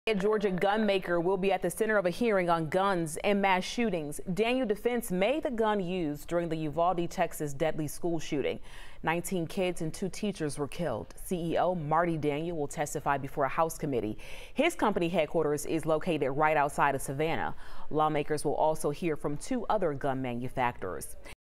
Georgia gun company will be at center of hearing on guns, mass shootings
Marty Daniel, CEO of Daniel Defense will testify.